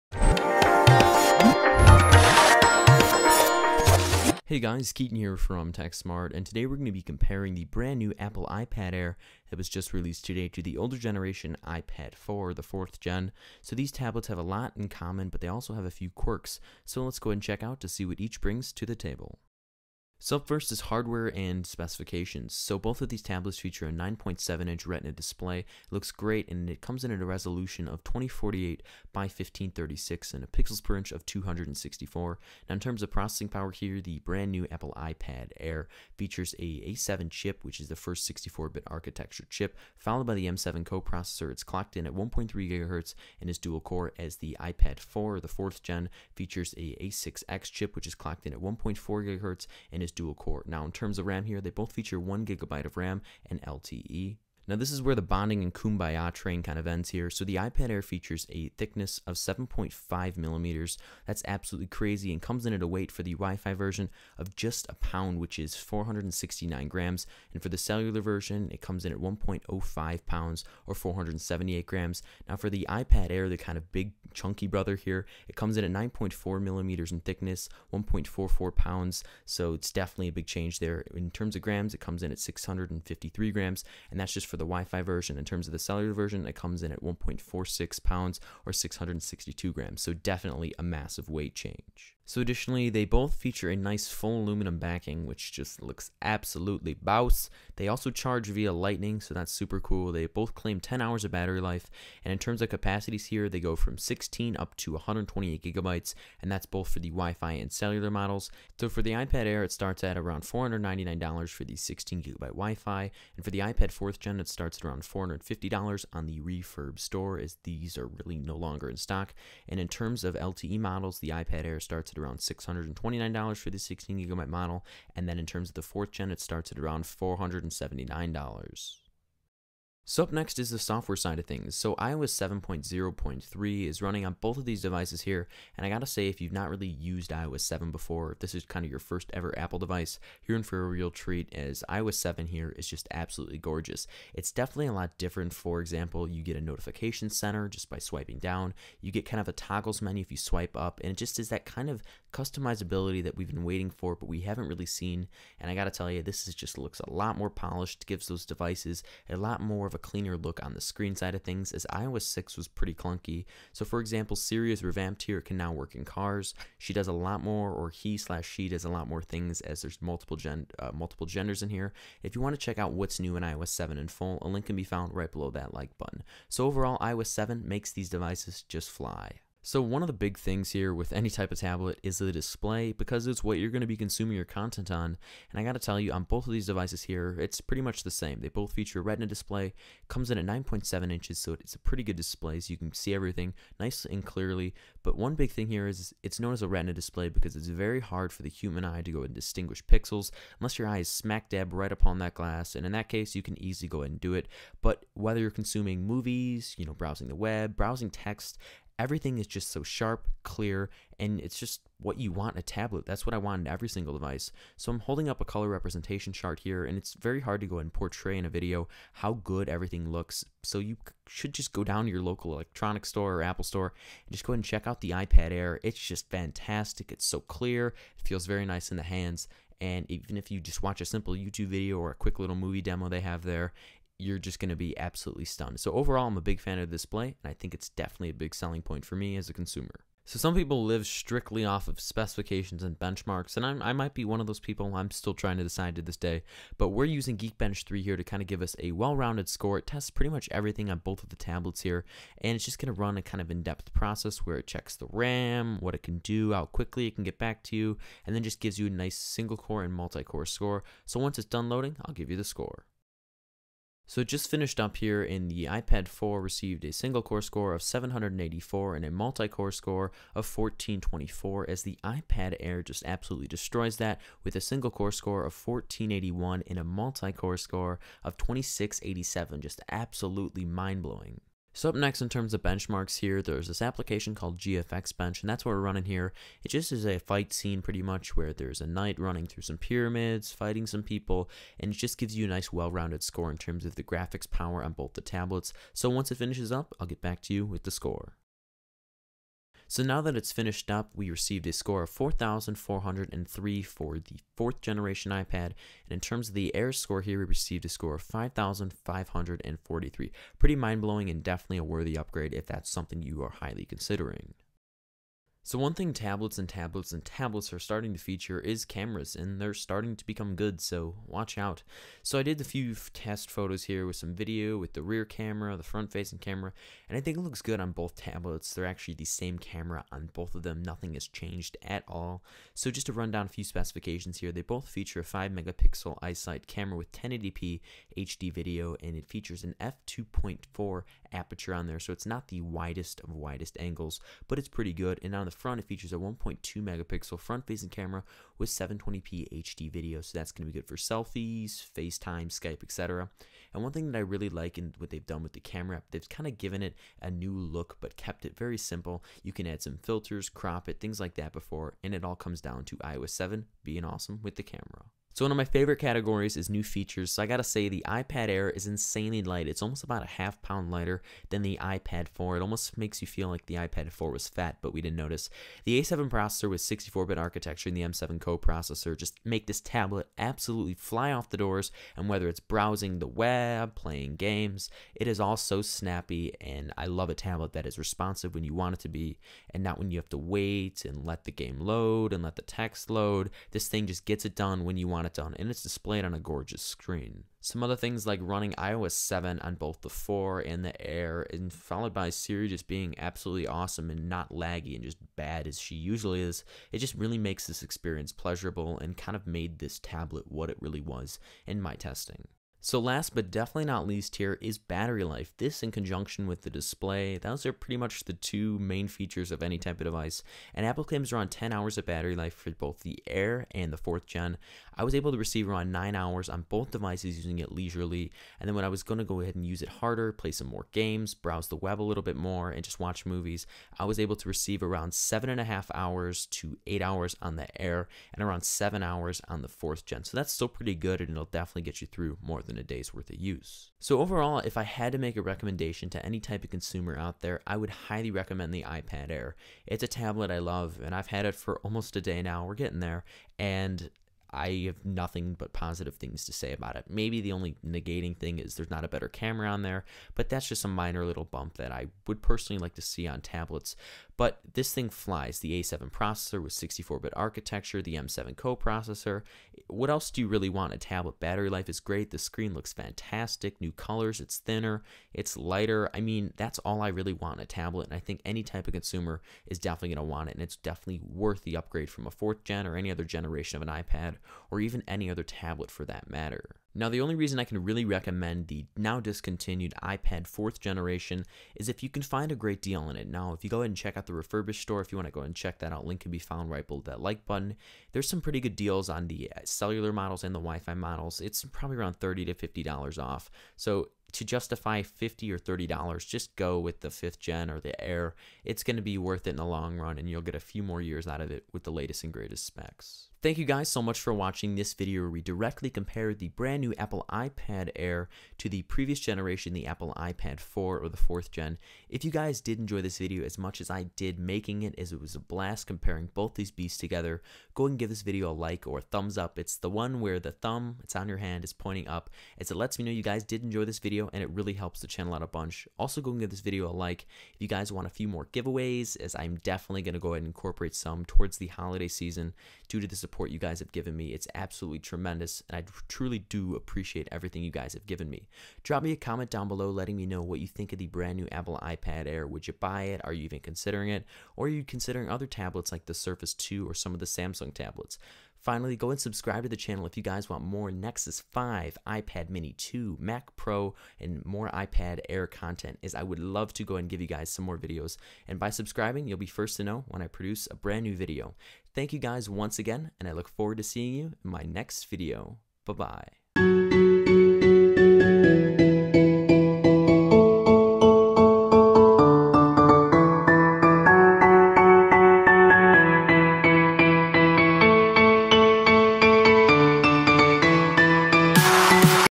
iPad Air vs iPad 4 - Full Comparison
In this video, we compare the newly released iPad Air to the older iPad 4. Specification differ these tablets, but software remains the same. We've compared the iPad Air vs Nexus 7 (2013) and will be coming the iPad Air vs iPad mini 2. What changes are noticeable? Lets find out!